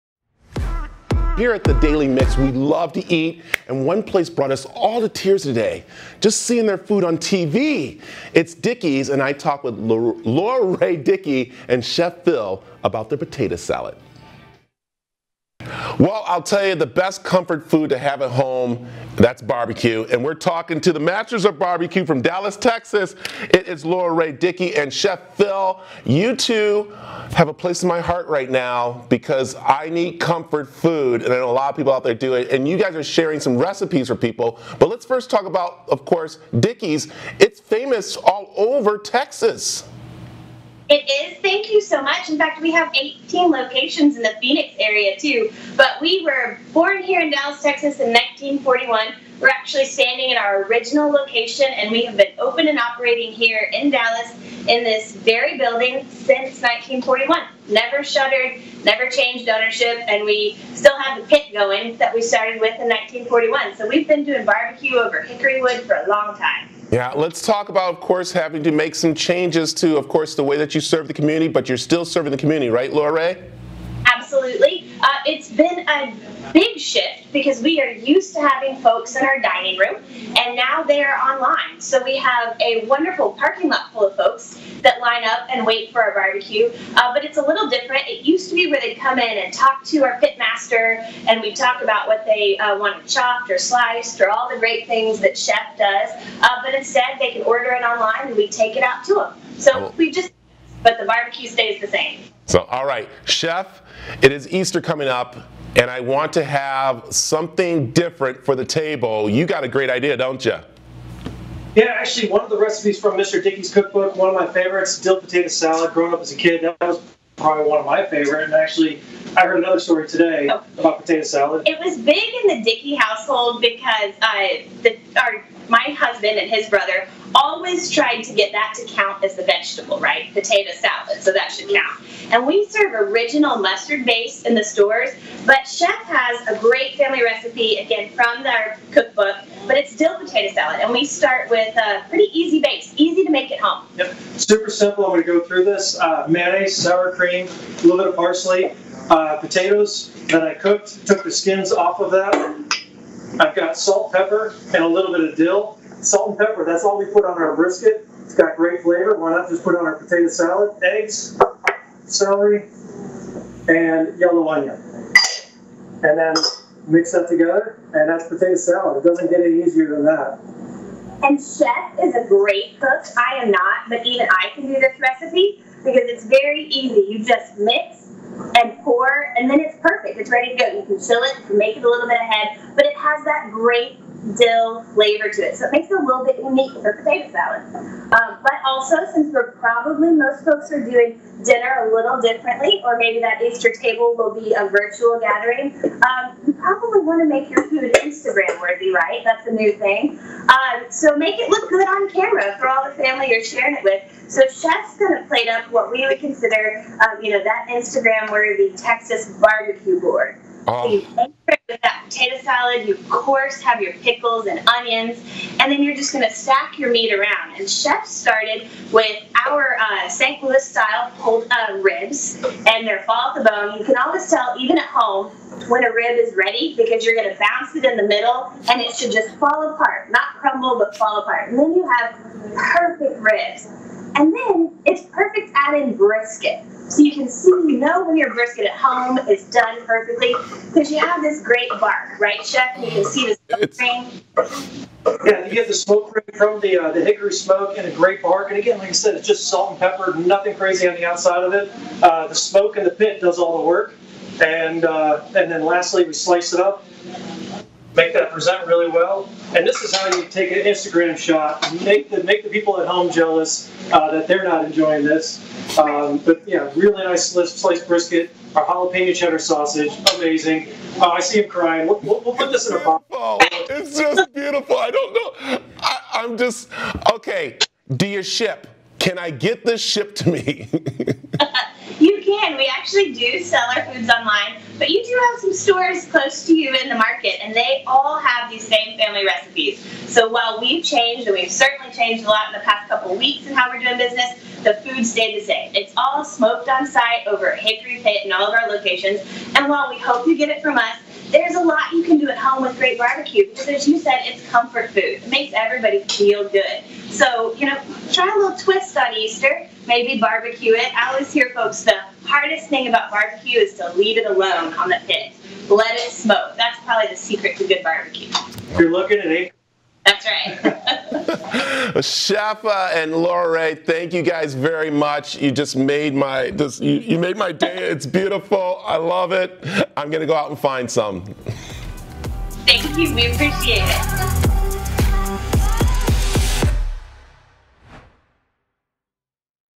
It’s Dickey's BBQ for Easter!
Dickey's Barbecue Pit is open and ready to help your family celebrate Easter with some hickory-smoked favorites.